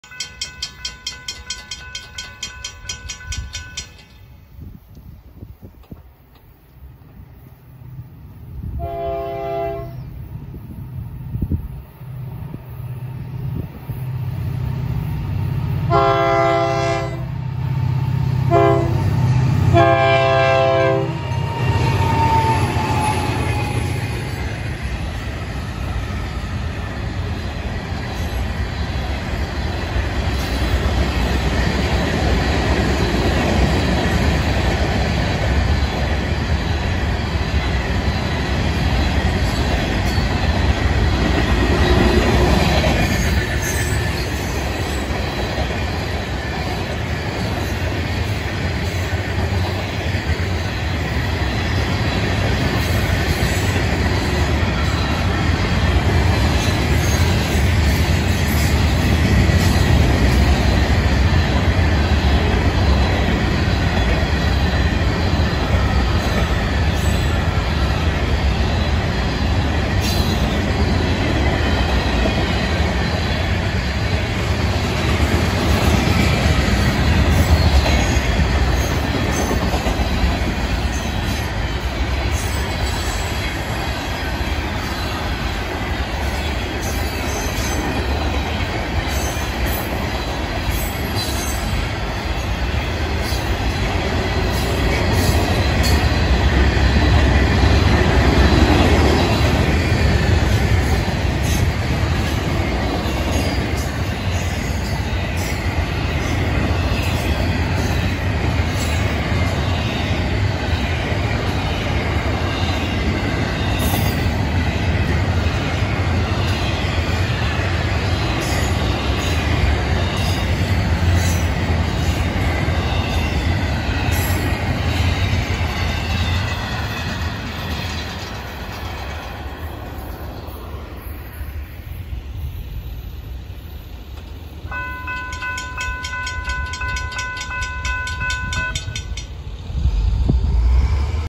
FEC TRAIN 103 @ 9:05 am 3-26-24
Southbound Fec engines 427&414 leading, sugar,tanks,gons,freight cars,Tropicana,& intermodal